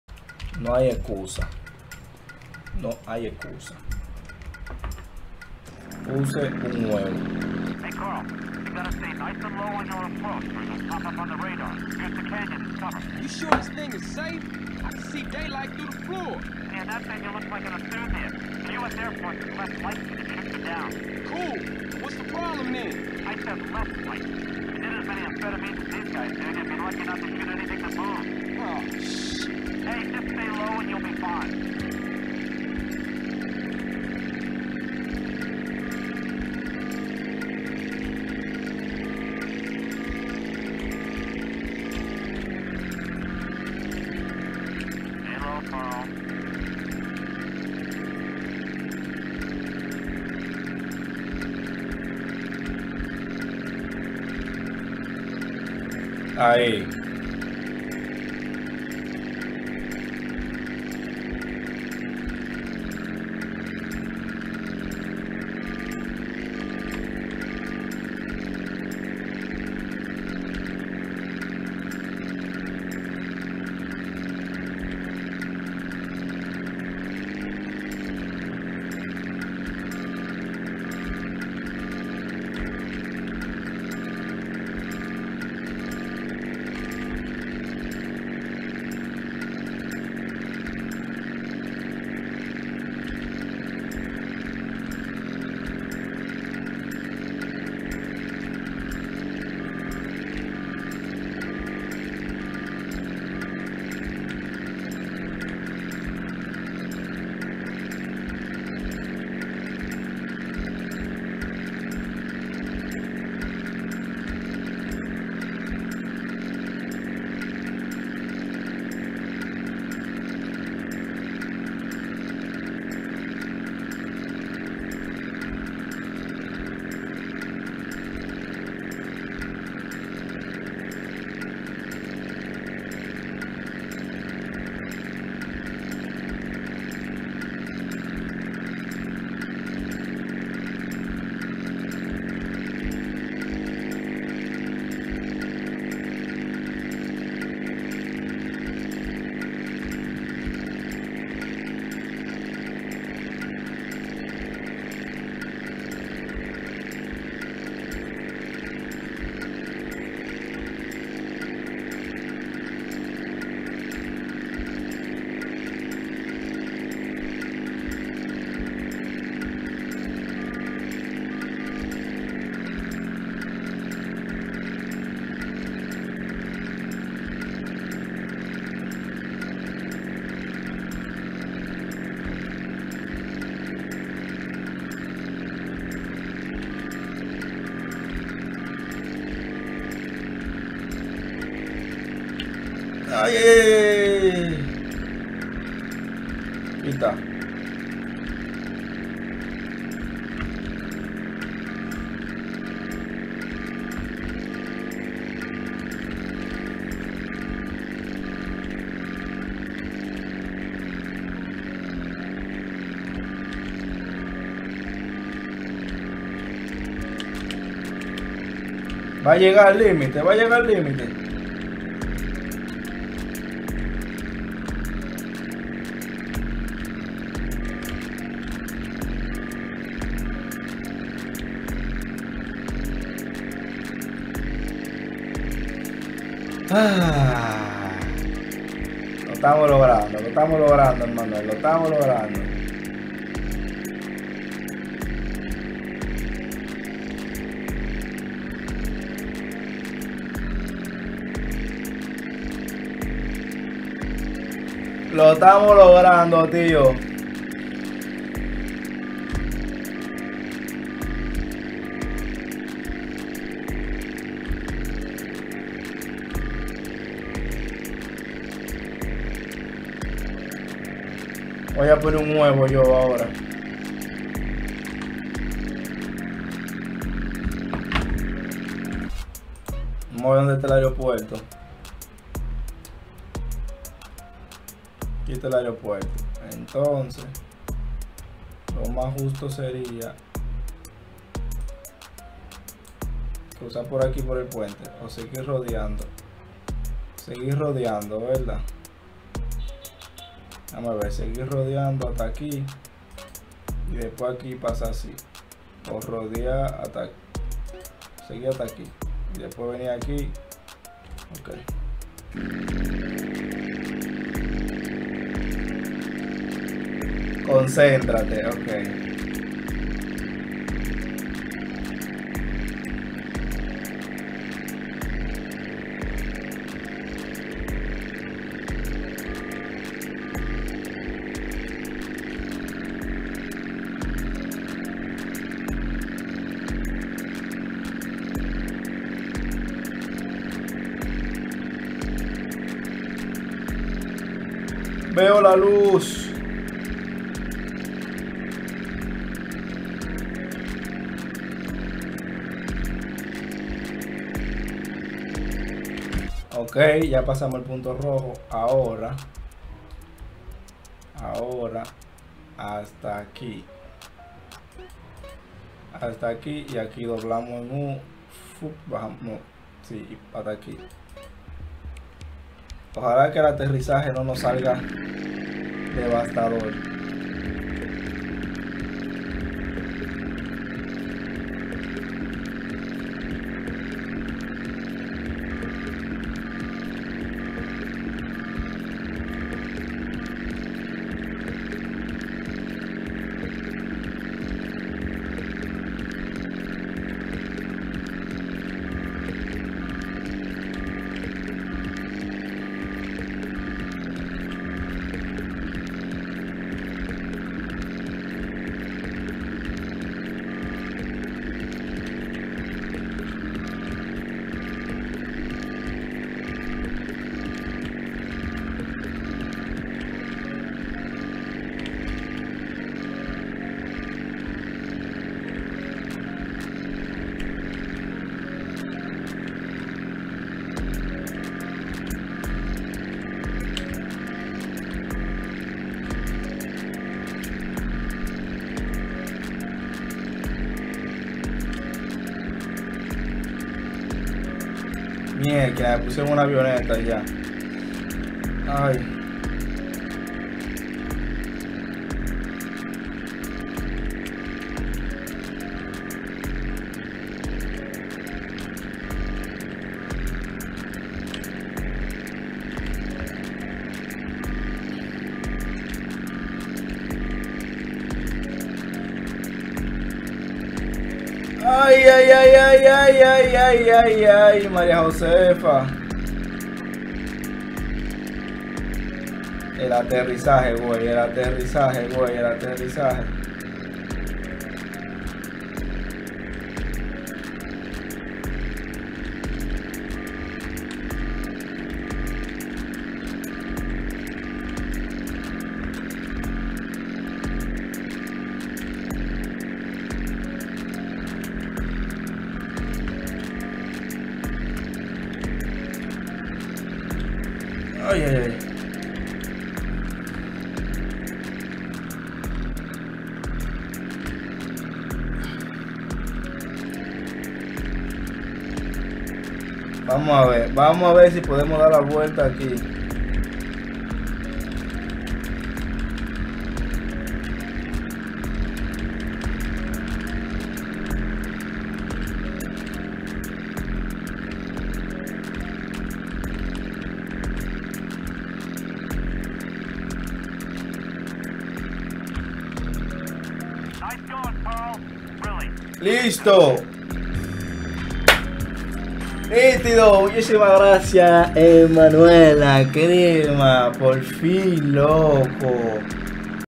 A SI SE PASA LA MISION DE LOS KILOS
Esta es una de las misiones mas difíciles, su propósito es que no pases,  por la ciudad en la avioneta, pero puedes rodear todo el mal, y el radal no te detecta... Espero la pases jajaja
Emmanuel saludos!